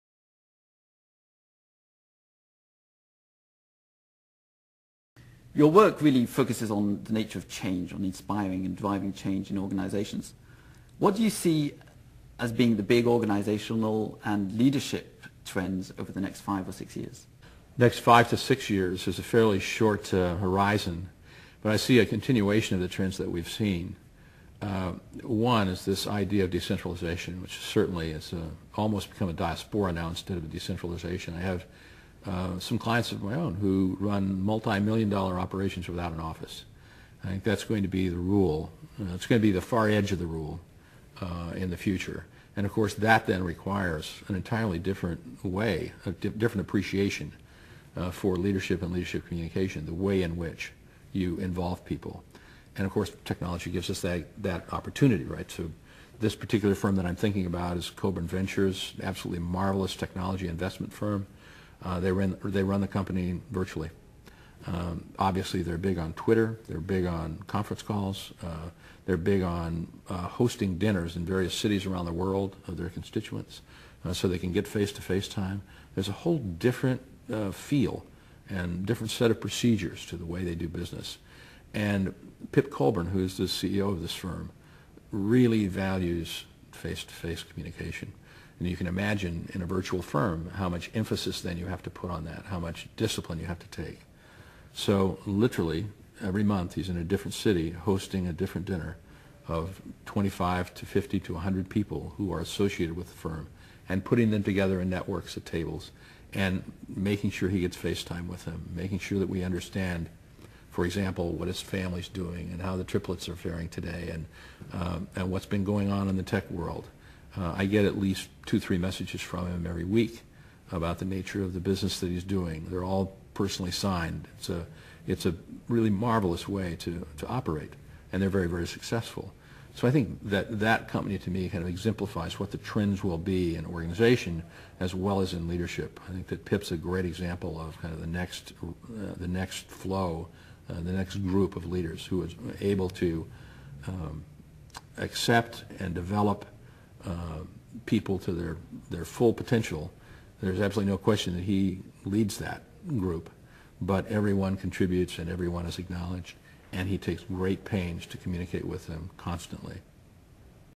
Leadership Communications in the Virtual Environment - Terry Pearce
Author, Professor and Coach Terry Pearce discusses how the current trend towards decentralization and virtual companies affects the leadership communication task of leaders. (c) BlessingWhite 2010